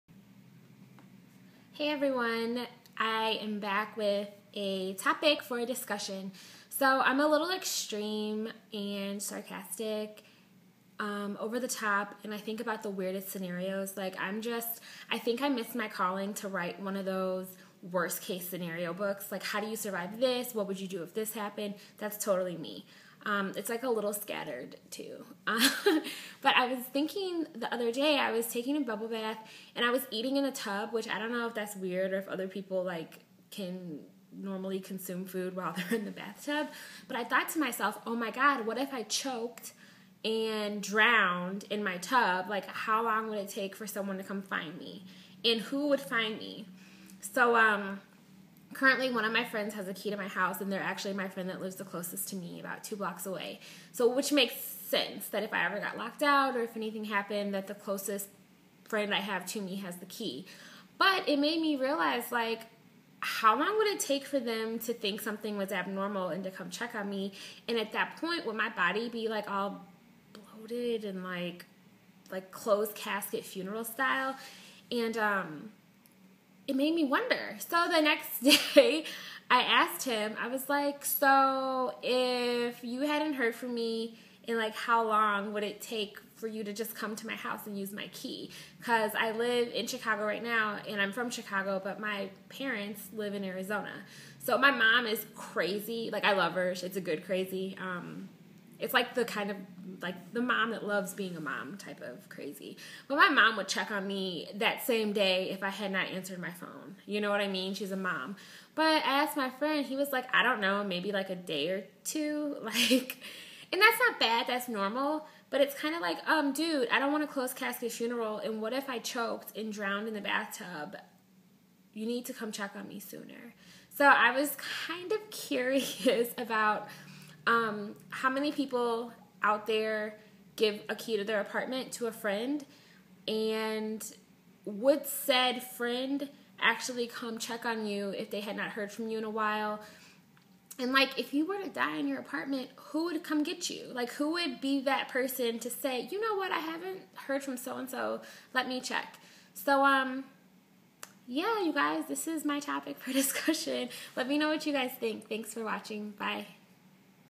Who would find your body?
Who has a key to your apartment and how long would it take them to find your body?